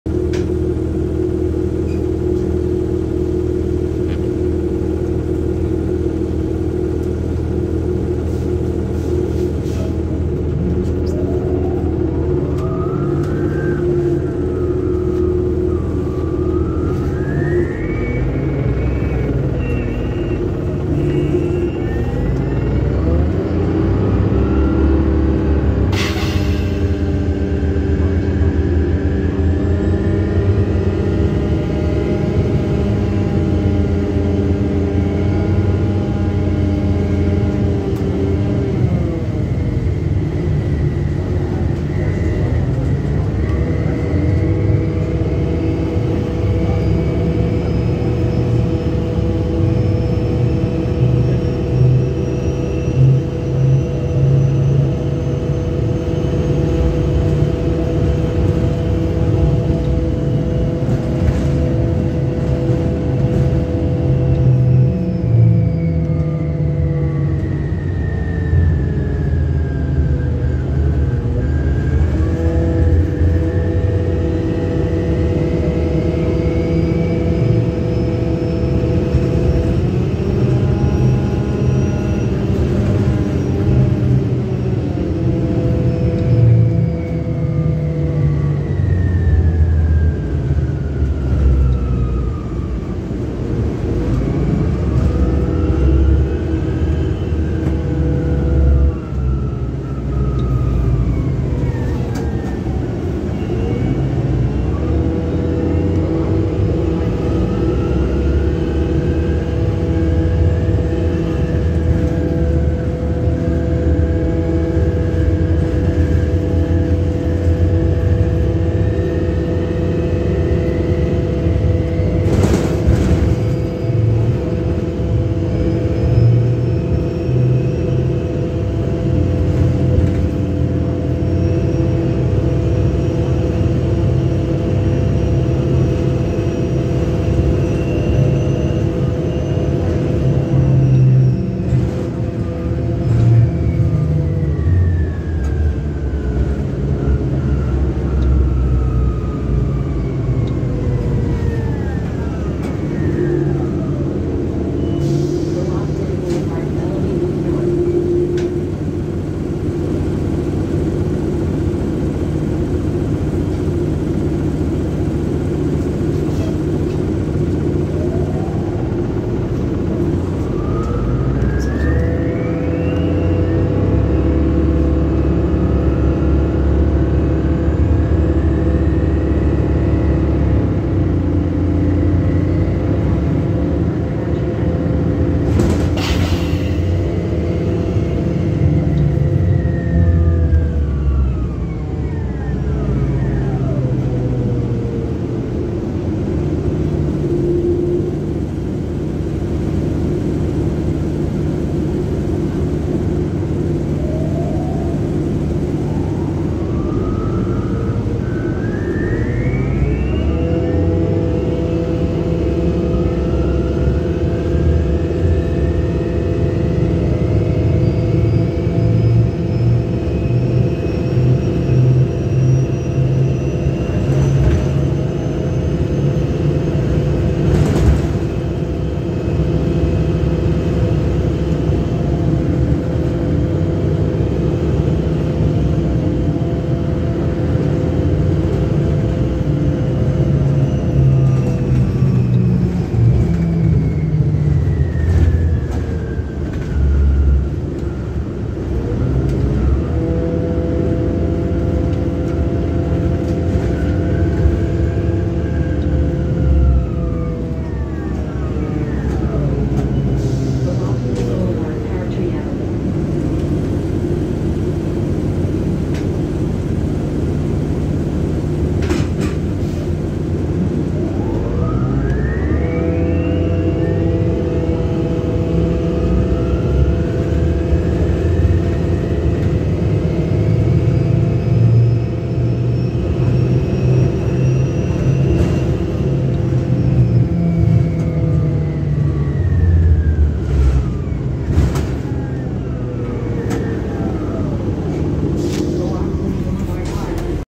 MTA Bus: 2009 Orion VII NG HEV #4036 [Audio Recording]
(Reupload) An audio recording of an MTA Orion VII Next Generation Hybrid bus #4036 on the Q50 Limited heading to Flushing Main Street. The recording was taken on March 13th, 2023 between Hutchison River Parkway on Einstein Loop North and Carver Loop #1 on Co-op City Blvd

Cummins IBS engine
BAE Hybridrive